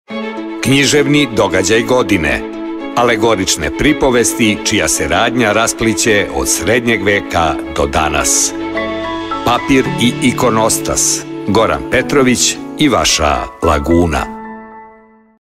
„Papir“ i „Ikonostas“ Gorana Petrovića
Roman Delta

Kakva je ovo zemlja po kojoj i svetac posrće?! Kakva je ovo zemlja gde svetac ne sme ni na tren da prispe, inače sa njegovog oreola zlato sastrugaše? Kakva je ovo zemlja iz koje se čak i kože odranih životinja trude da pobegnu? Kakva je ovo zemlja u kojoj iguman ne prima ikonu na prenoćište? Kakva je ovo zemlja koja vara namernike, gde se sela premeštaju, napuštena troskišta se smradno puše, ljudi gase zvezde-žežnice?! Kakva je ovo zemlja gde se stena, tvrdi kamen pod mekom, nežnom mahovinom – samo pravi da nikoga neće da ubije?!

Srbija za vladavine despota Stefana Lazarevića, početak XV veka. Despotov prestoni dvor u Beogradu, koji tek što je izgrađen, treba opremiti prefinjenim baldahinima iz Dubrovnika, debelim perinama iz Pešte, neugasivim kandilima iz Soluna, svetozarnim ikonama sa Svete Gore… Donosiće ih nosači peške, natovarene na mazgama ili prevožene lađama, jedino ikonopisani sveci, njih stotinu, odlučuju da krenu put srpske prestonice sami, bez ičije pomoći.

Da ikone mogu da lete i putuju, zna se odavno, ali u ovoj fantastičnoj pripovesti Gorana Petrovića one će doživeti i mnoge nevolje, presretaće ih i Turci, i razbojnici, i morske nemani, i vremenske nepogode, dočekivati i klanjati im se vernici, mnoge će se zauvek izgubiti, a u despotov dvor stići će samo jedna, oštećena i gotovo neprepoznatljiva, pa ni despotov savetnik Konstantin Filozof neće razaznati o kojem se svetitelju radi…

Ikonostas je nova priča, novi „tok“ u Romanu delti Gorana Petrovića, univerzumu alegoričnih pripovesti čija se radnja raspliće od srednjeg veka do dana koje smatramo našim.
Roman Delta

Papir iz grada Amalfija, nekada slavne pomorske državice, bio je tim vredniji što za njegovo posedovanje nije bilo dovoljno da kupac ima čime da ga plati. Kada su i od koga vlasnici manufaktura još tokom XIII veka naučili da prave najfiniji papir od iznošene odeće nije u potpunosti razjašnjeno, ali oni ne samo da su se tokom narednih dve stotine godina veoma obogatili već su postali i uticajni…

Nadaleko proslavljeni esnaf Congrega dei Cartari iz italijanske luke Amalfi, čuveni je proizvođač najfinijeg papira u Evropi, ali je poznat i po tome što pomno bira kome će ga prodati. Zato napuljska kraljica Đovana II, inače poznatija kao Đovana Nezasita, odlučuje da lično preduzme pohod u ovaj mediteranski grad da bi kupila papir za ljubavno pismo izvesnom Pandolfu Piskopu, do čije joj je naklonosti naročito stalo. U pohod kreću i brojni pripadnici kraljičine svite, njena posluga, vojska, lična straža i drugi pratioci, a kao najvažniji putnici – napuljski hroničari i ondašnji pesnici sa zadatkom da na naročitom papiru iz Amalfija uobliče kraljičina osećanja, a time i ovekoveče ovo slavno putovanje s početka XV veka.

Priča o jednom događaju u renesansnoj Italiji poslužila je Goranu Petroviću da svoju novu knjigu, raskošno ispripovedanu povest Papir, uobliči u dalekosežnu i duhovitu metaforu o smenama života i smrti, o tajnama pisanja i stvaranja uopšte.

Papir je prva priča, „prvi tok“ u Romanu delti Gorana Petrovića, univerzumu alegoričnih pripovesti čija se radnja raspliće od srednjeg veka do dana koje smatramo našim.